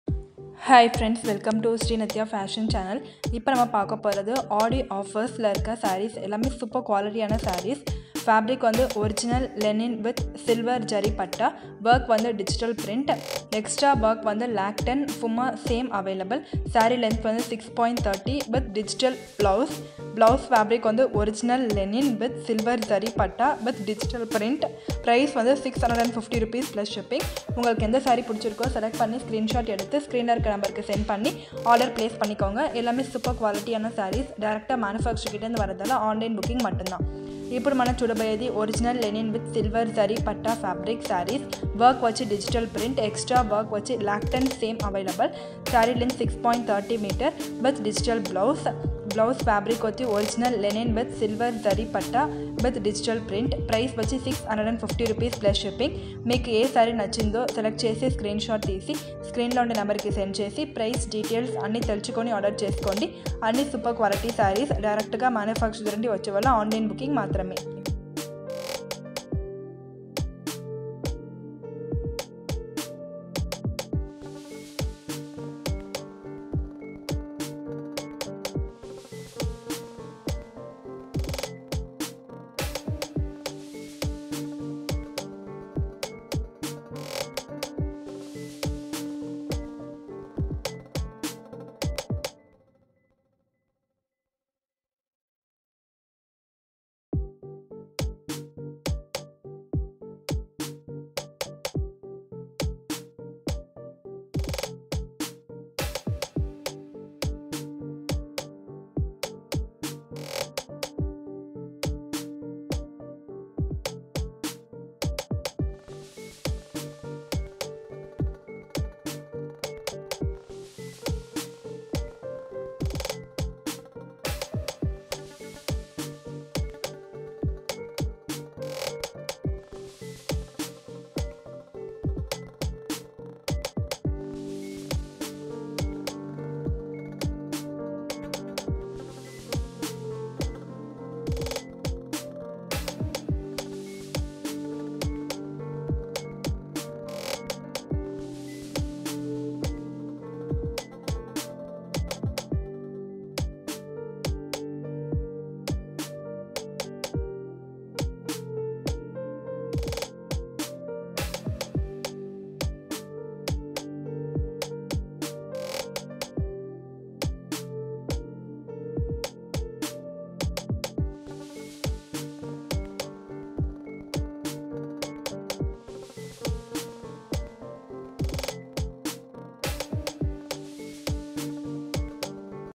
Original Linen With Silver Jari Patta Sarees at Rs .650+$ (‎@Sri Nithya Fashion Boutique )
Original Linen With Silver Jari Patta Sarees available at Rs .650+$ (@srinithyafashionboutique7932 )

*Fabric* - Original Linen With Silver Jari Patta.

*Work* - Digital Print.

*Extra Work* - Latkan (Fumka) Same available.

*Saree length* - 6.30 Cut (with Digital blouse).

*Blouse Fabric* - Original Linen with silver jari patta with Digital print.

*Rate  - 650+ship